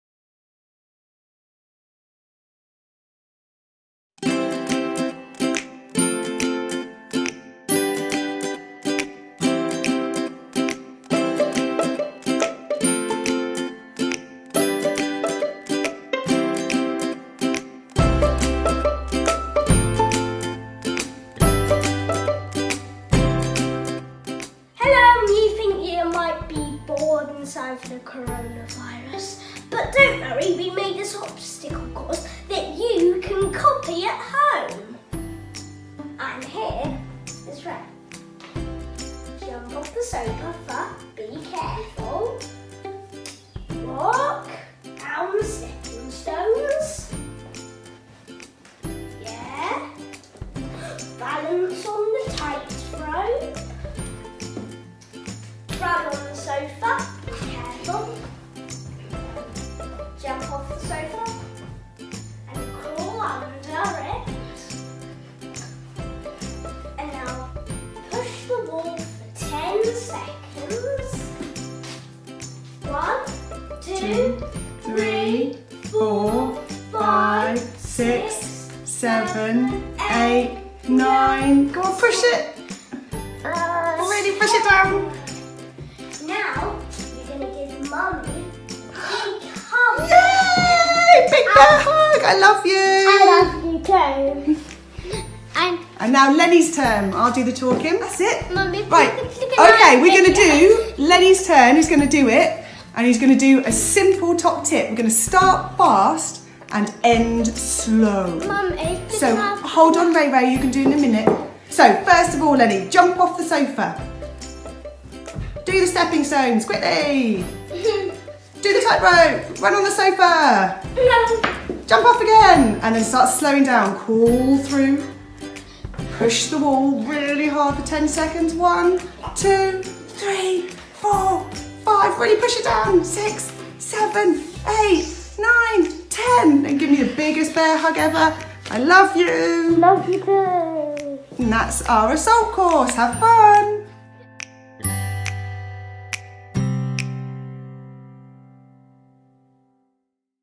Obstacle Courses
A great way to keep children regulated (and entertained!) is to get them to do obstacle courses. They can plan their own, write it down, test it, change it, most importantly have fun with it. My boys had a go, have a look. Try and encourage your children to start with fast activities and slow down the pace towards the end, ending in calming activities.

Some ideas from ours:
We have included: alerting activities - these wake up the brain; organising activities - these get the body and brain working together; calming activities - most important at the end to support regulation and not leave children 'fizzy'.
- Jumping - alerting - ideally jump into a crash mat/pile of pillows
- Stepping stones and tightrope - organising activities - you could also do star jumps and squat jumps, alternating between the two to really get the brain in gear
- Run on sofa - alerting - or you could run on the spot, or use a hoola hoop
- Crawling - begin to calm the body
- Push the wall - calming - get your child to push as hard as they can, as if they were trying to push it down. Or they could do press ups if older.
- Big bear hug - calming, wonderful co-regulation. If your child prefers not to hug then you could squash them with a pillow whilst they lie on their tummy (not over their head) or give them a hand massage if they prefer to sit up.

Have fun!